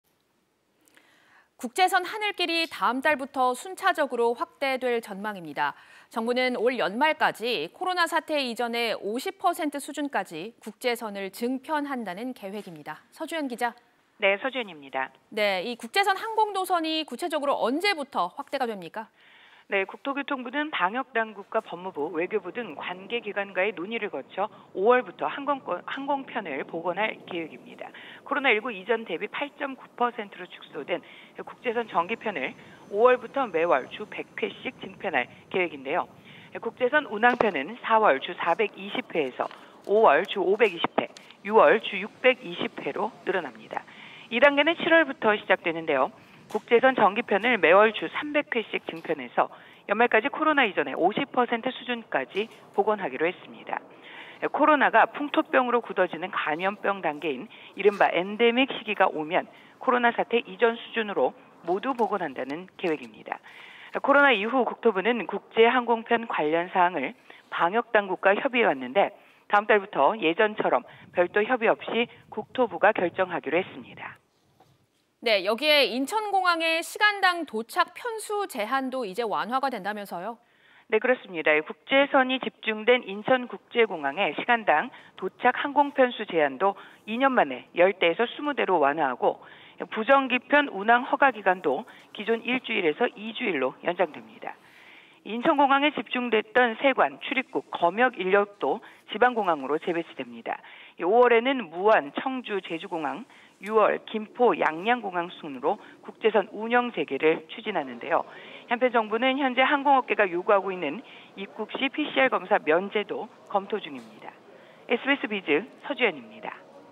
5월부터 해외여행 하늘길 넓어진다…연내 50% 회복
[앵커]

국제선 하늘길이 다음 달부터 순차적으로 확대될 전망입니다.

정부는 올 연말까지 코로나 사태 이전의 50% 수준까지 국제선을 증편한다는 계획입니다.

서주연 기자 연결합니다.

국제선 항공 노선이 언제부터 확대되는 건가요?

[기자]

네, 국토교통부는 방역당국과 법무부, 외교부 등 관계기관과의 논의를 거쳐 5월부터 항공편을 복원할 계획입니다.